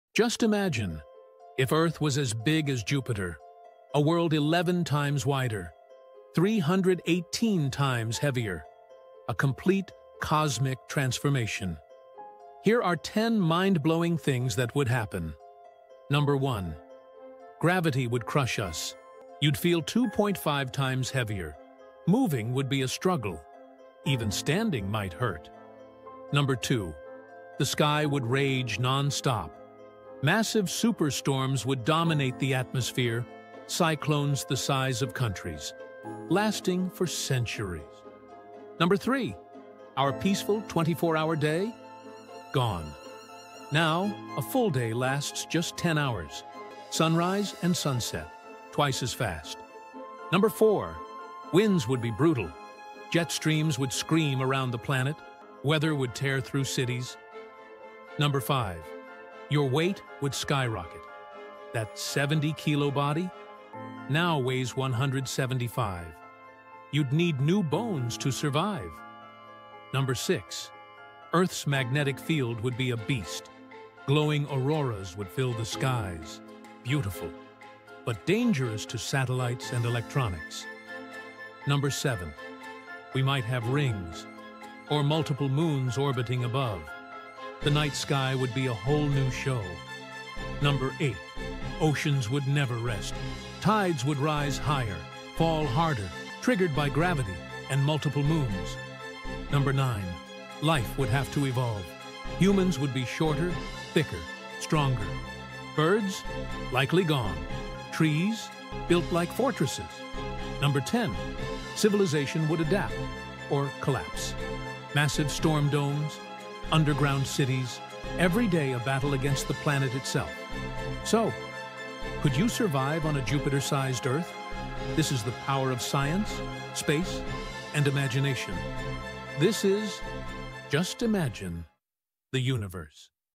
Just Imagine: If Earth Was as Big as Jupiter 🌍🔥 | 10 Mind-Blowing Facts
🌍 Just Imagine if Earth Was as Big as Jupiter...
What would happen if our small blue planet suddenly grew to the size of a gas giant?
Stronger gravity, endless storms, shorter days, multiple moons — this is not science fiction... it’s science imagination.

In this episode of Just Imagine: The Universe, we explore 10 insane things that would happen if Earth became as massive as Jupiter. From crushing gravity to glowing auroras and sky-shattering winds, this cosmic scenario will blow your mind.

👽 Could life survive?
🌪 Would Earth still be habitable?
🌌 Or would it become an alien world beyond recognition?

🎙️ Sit back and enjoy this epic journey through imagination, space science, and futuristic possibilities.